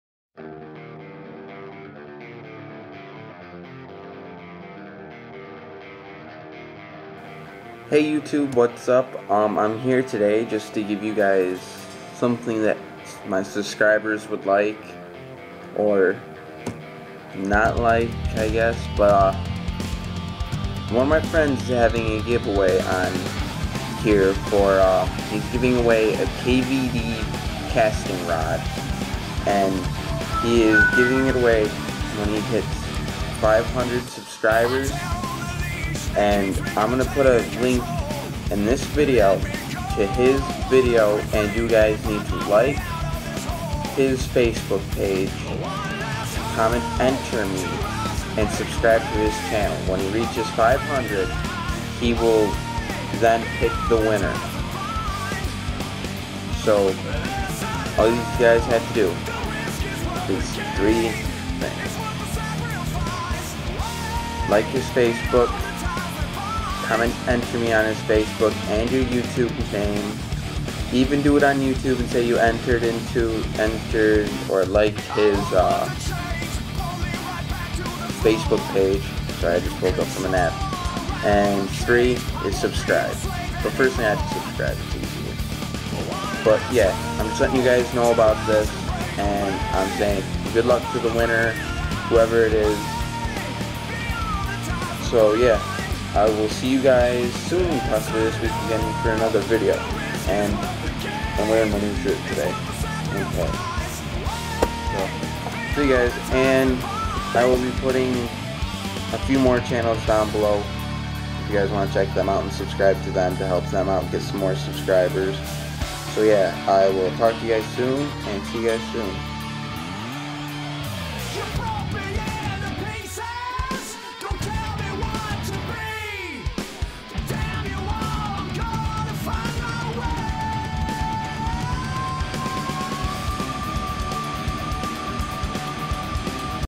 *fixed link* Helping my friend get Subscribbers read description
hey guys im hekping my friend philly bassin' with getting subscribers he is having a give away too :D hope you enjoy it!! heres link to his channel" " hope you enjoy! keep watching for my unboxing of the give away i wont from him! the link to the video "